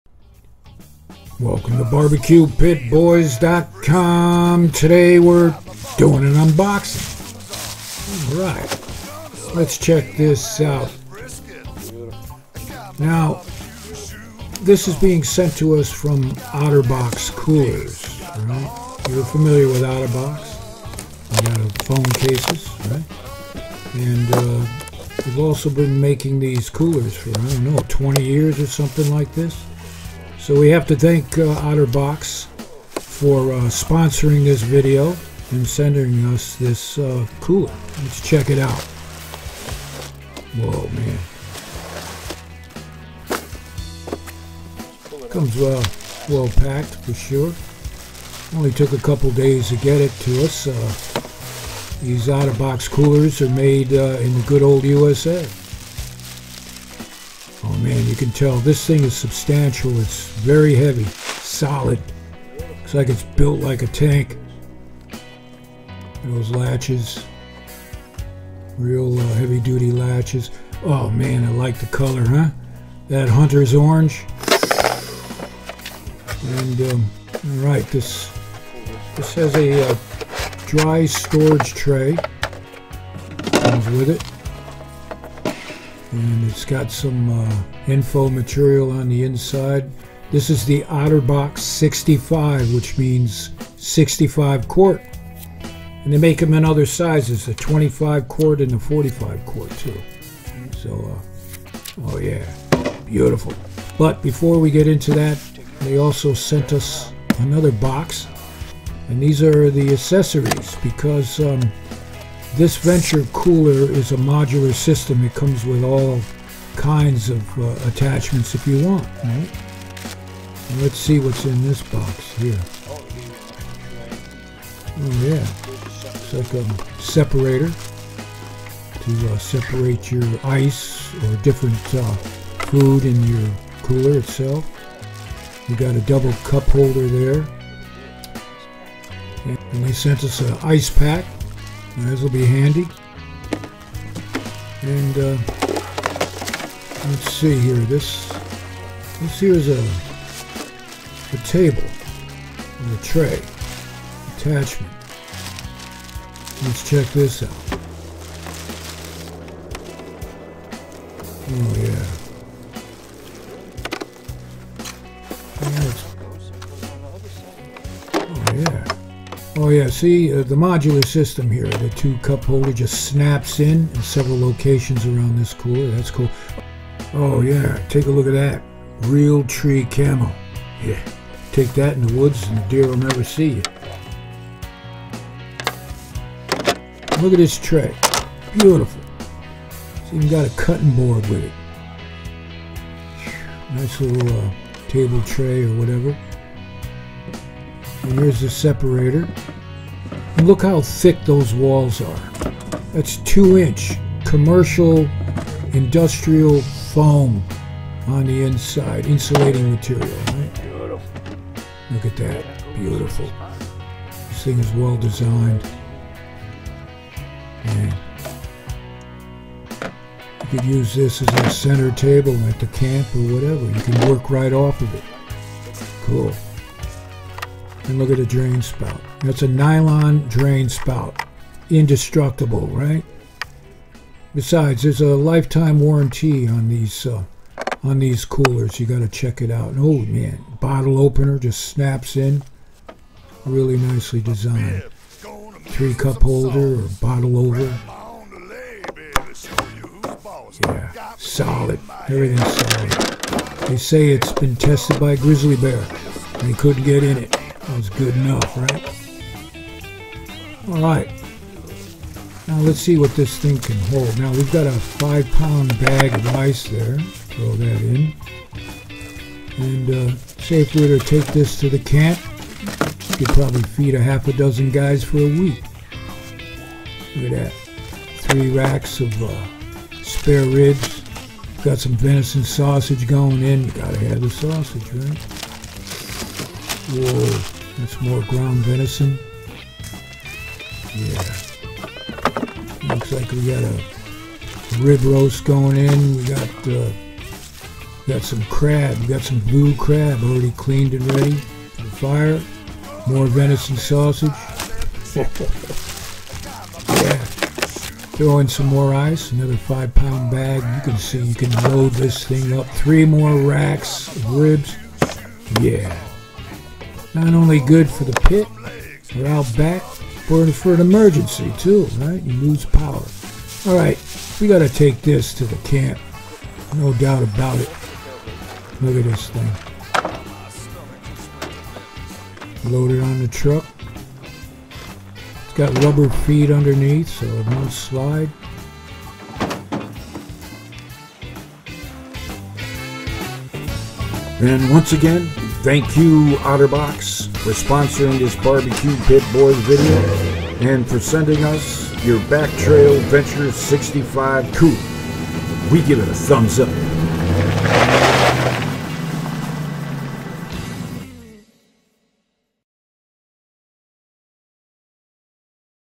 [UnBoxing] OtterBox Cooler
OtterBoxCoolersasked us to review their Back Trail Venture Cooler with attachments here at the Pit. Much obliged. We appreciate their support by sponsoring the BBQ Pit Boys and this video.